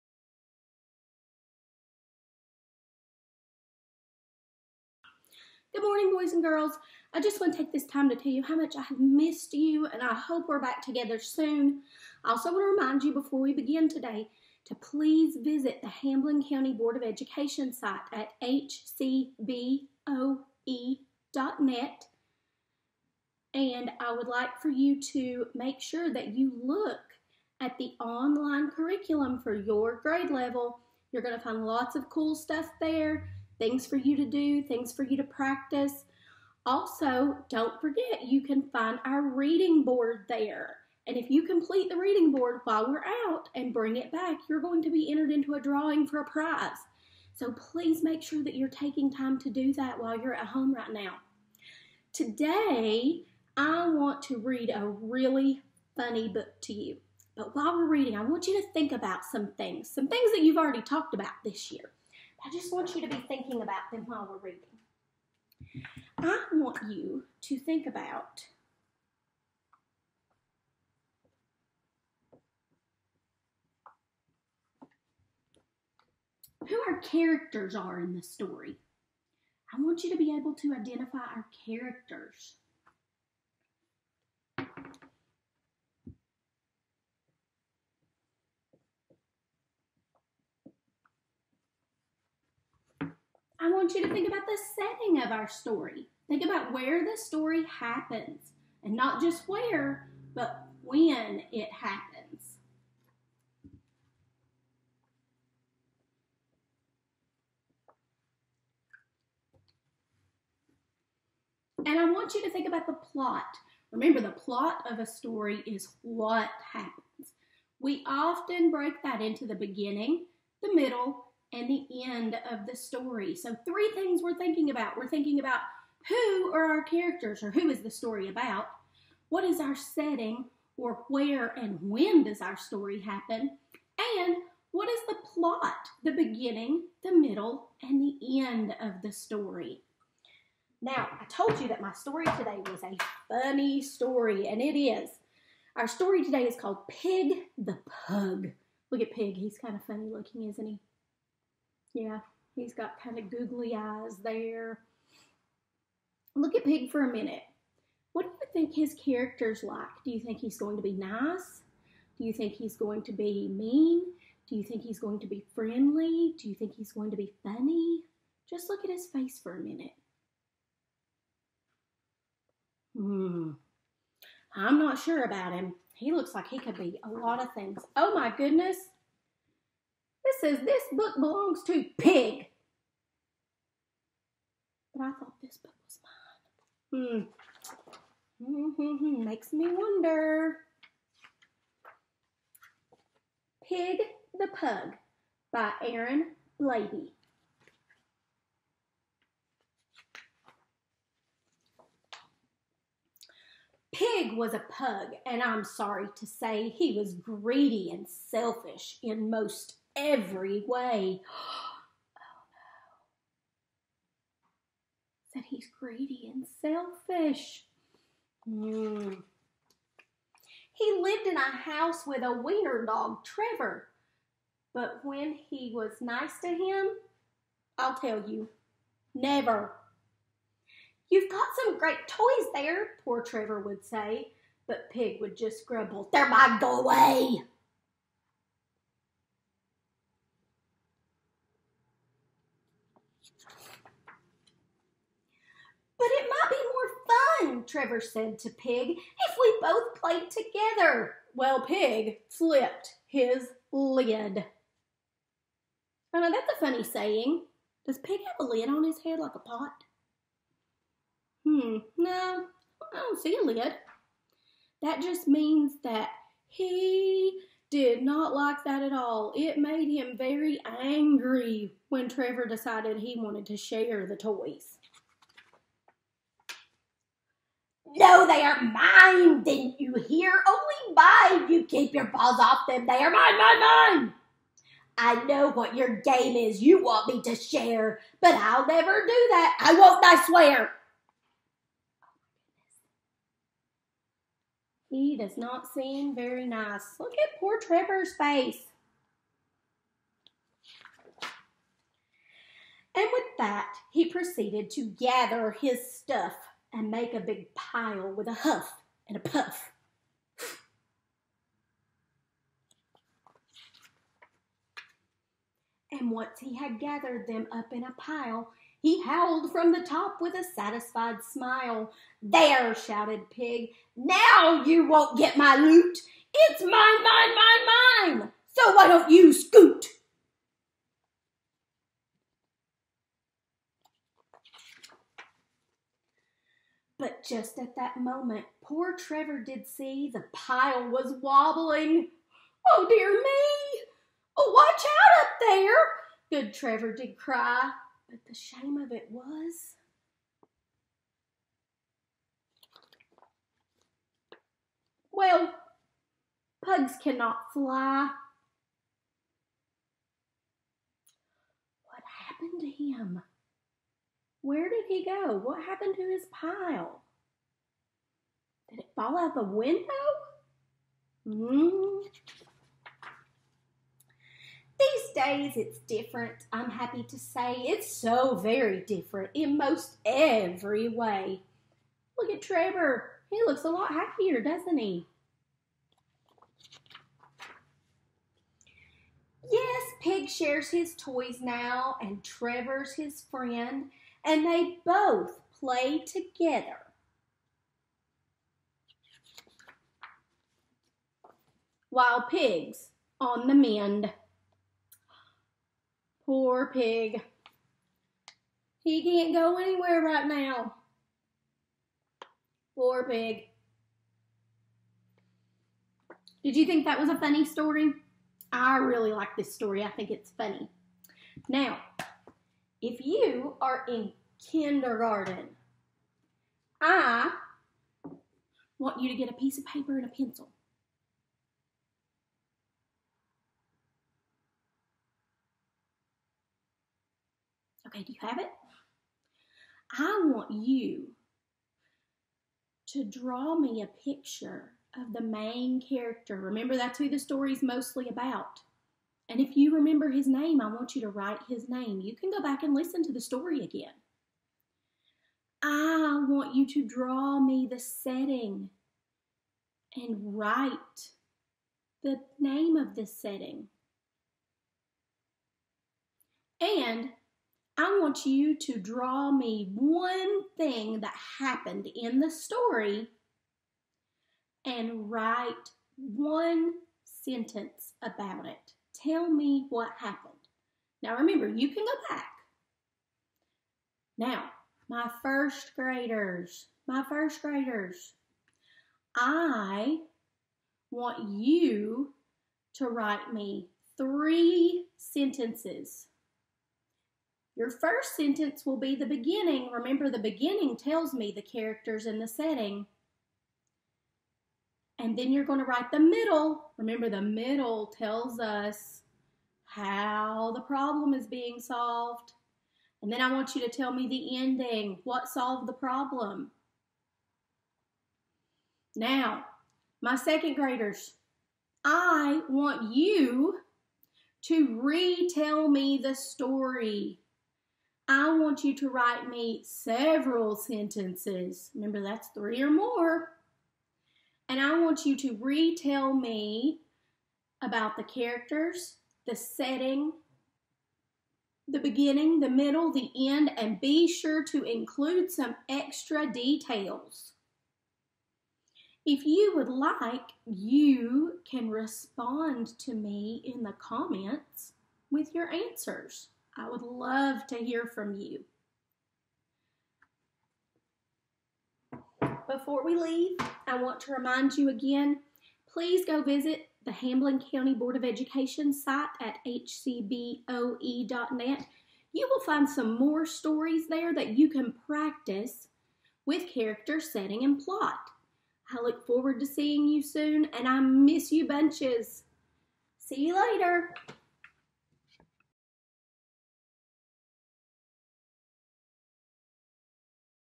K - 2 Library - Week 1
We will be reading Pig the Pug with permission from Scholastic Books.
As I am reading, I would like you to think about our characters, setting, and plot.

After listening to the story:
Kindergarten - Draw a picture of the main character and write the name, draw a picture of the setting and label it, and draw one thing that happened in the story and write a sentence about it.
First Grade - Write three sentences telling the beginning, middle, and end. Remember, the beginning tells the characters, setting, and problem. The middle tells how the characters try to solve the problem, and the end tells the solution.
Second Grade - Retell the story in a paragraph. Remember this should be several sentences including the beginning, middle, end, and all the important details about the characters and the problem.